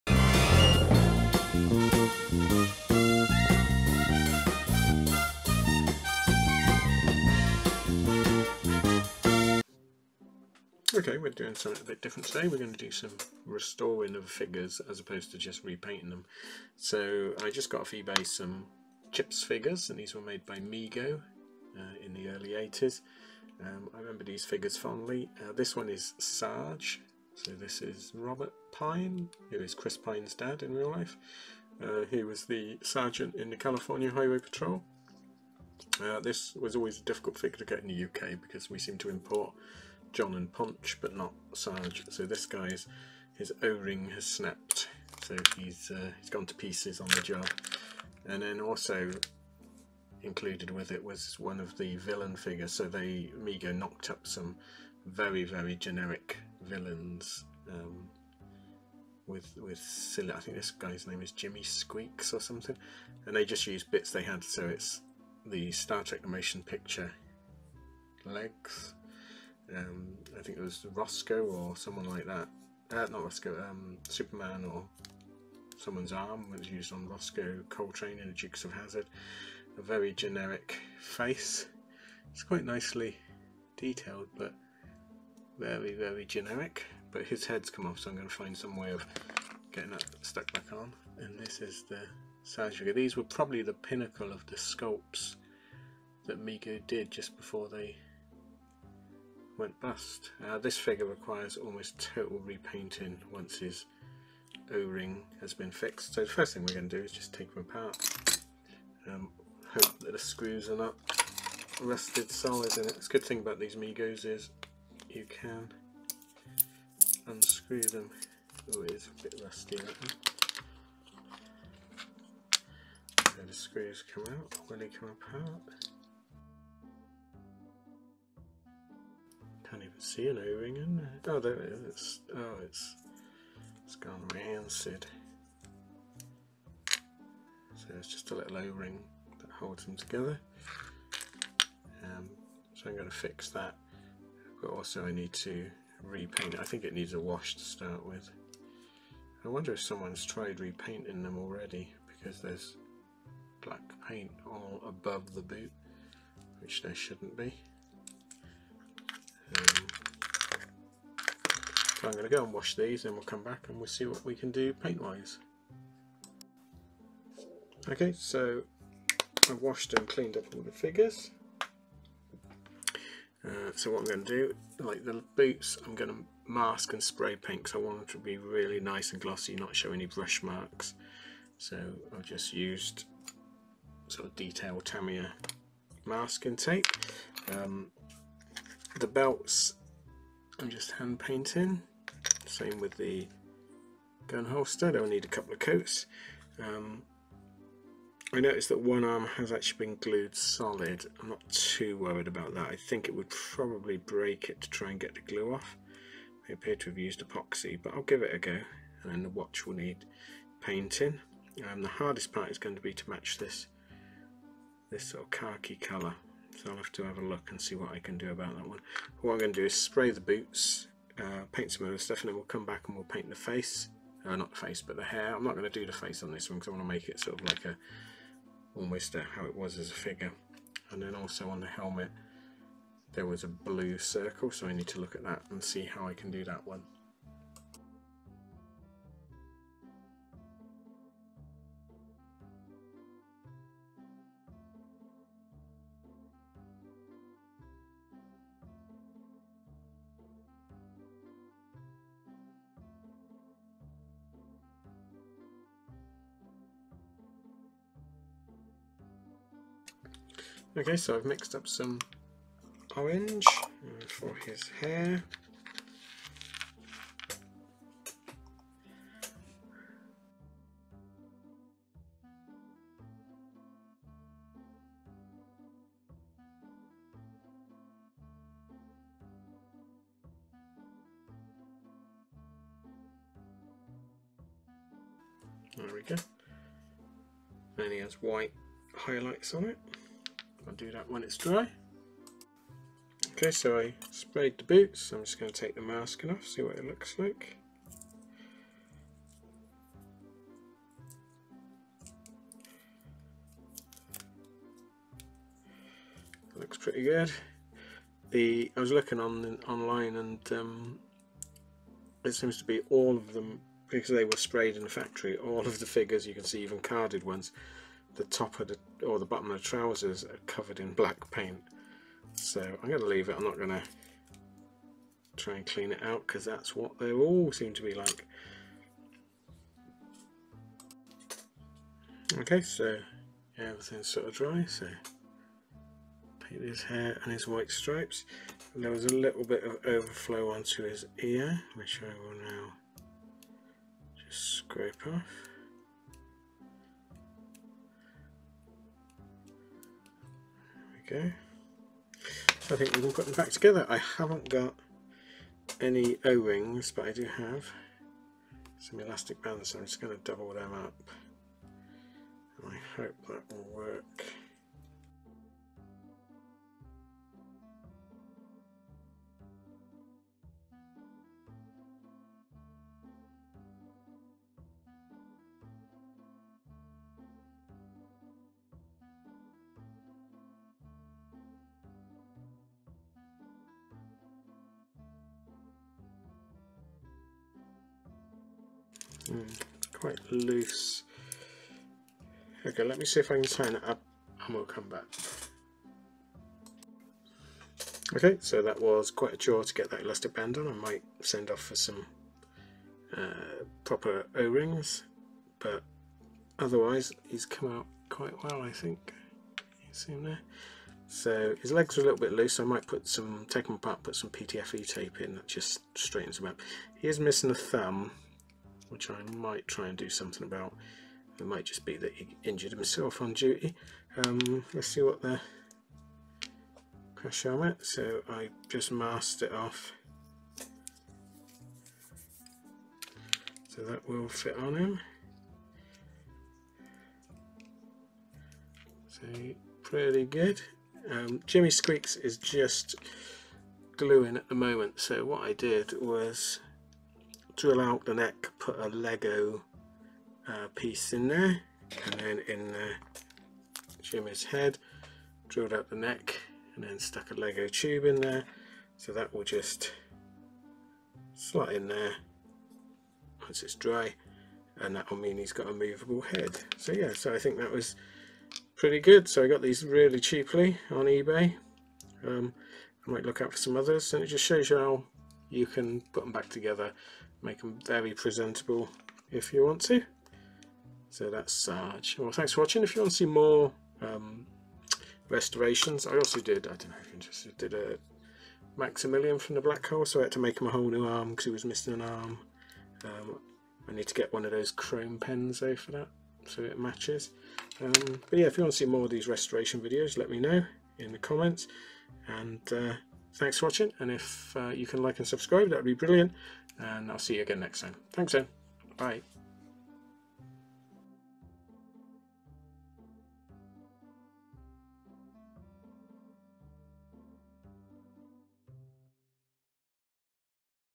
Restoring Mego C.H.i P.s figures
Restoring a couple of old Mego 3 3/4 Chips figures